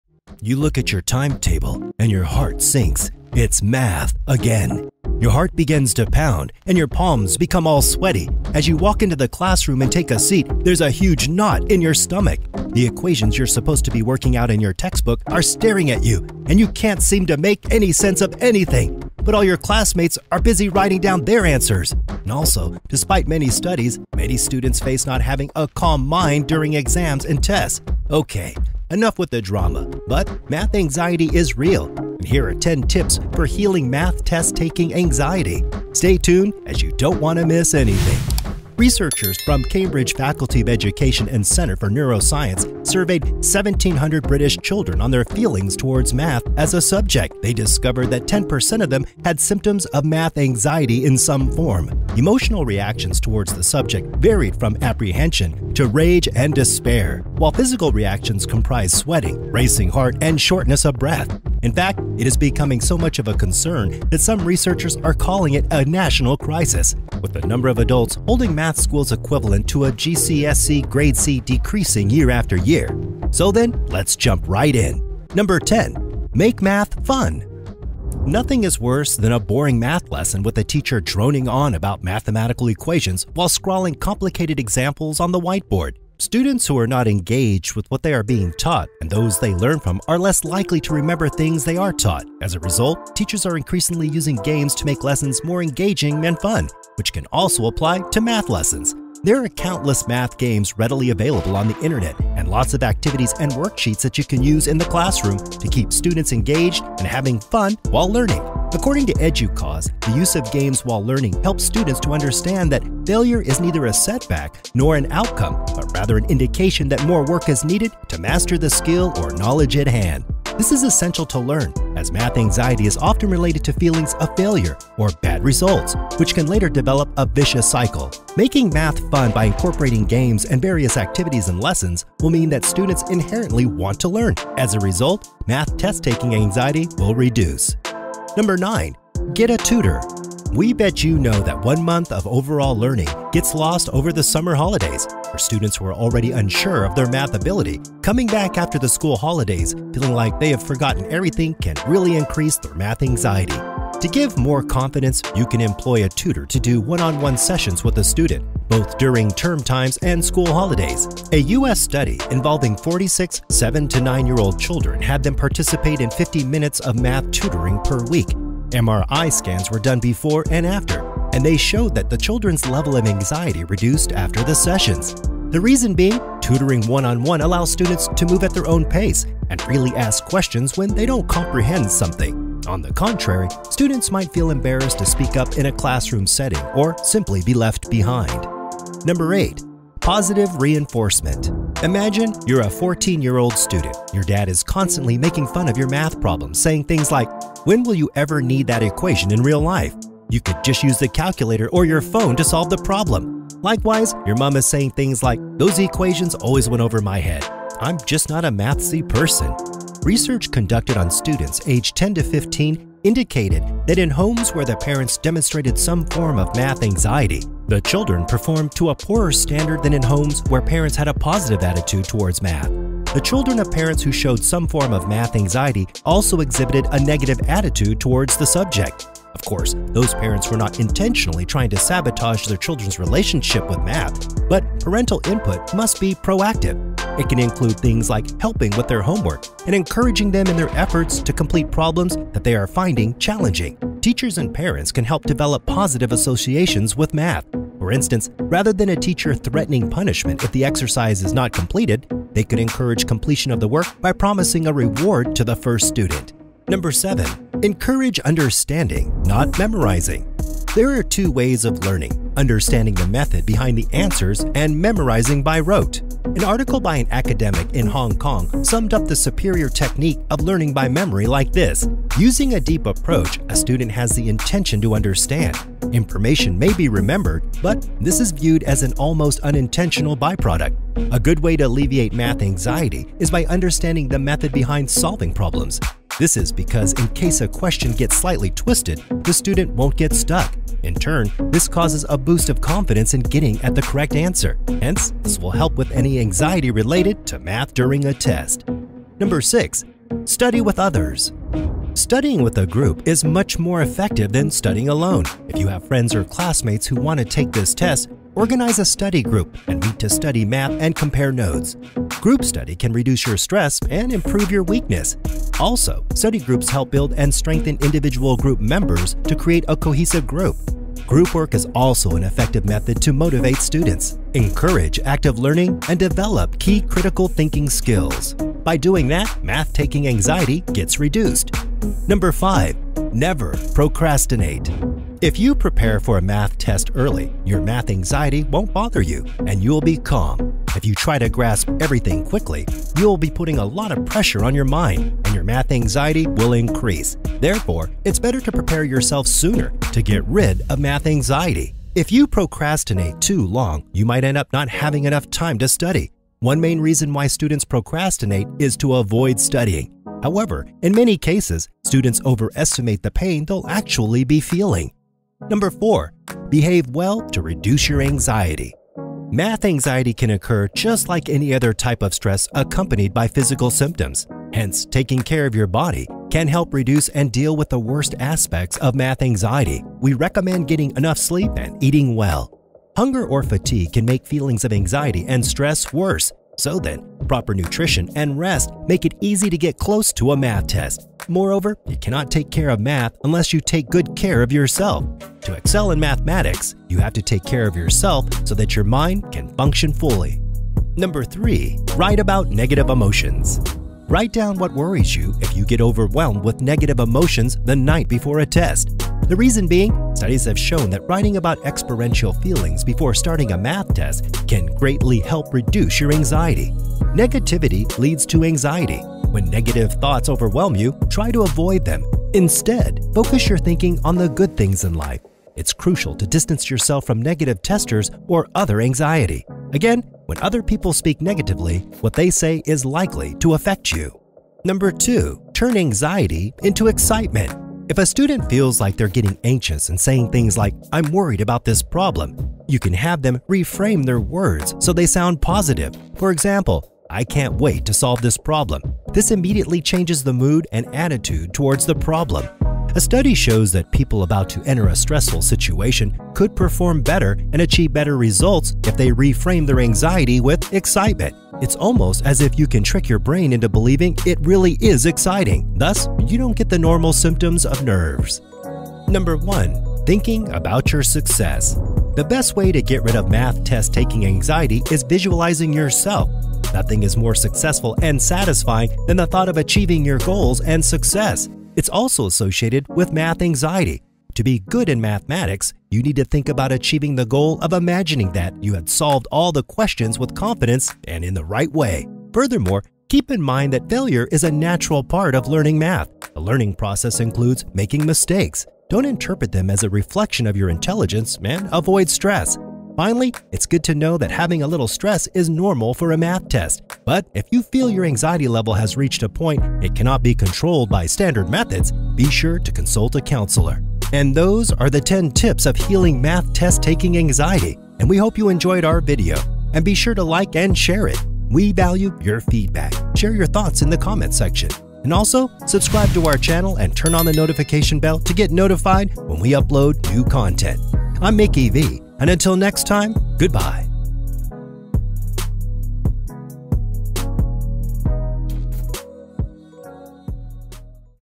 Top 10 Tips For Healing Math Test Taking Anxiety
You look at your timetable, and your heart sinks. It’s math, again. Your heart begins to pound, and your palms become all sweaty. As you walk into the classroom and take a seat, there is a huge knot in your stomach. The equations you are supposed to be working out in your textbook are staring at you, and you can’t seem to make sense of anything, but all your classmates are busy writing down their answers.

Also, despite many studies, many students face not having a calm mind during exams and tests.

Okay, enough with the drama, but math anxiety is REAL. Here are ten tips for healing math test-taking anxiety.

Researchers from Cambridge’s Faculty of Education and Centre for Neuroscience surveyed 1,700 British children on their feelings towards maths as a subject. They discovered that 10% of them had symptoms of math anxiety in some form. Emotional reactions towards the subject varied from “apprehension” to “rage and despair,” while physical reactions comprised sweating, racing heart, and shortness of breath.

In fact, it is becoming so much of a concern that some researchers are calling it a “national crisis,” with the number of adults holding math skills equivalent to a GCSE grade C decreasing year after year.

Do You Have Math Anxiety?
Get the tablet and products I use for math here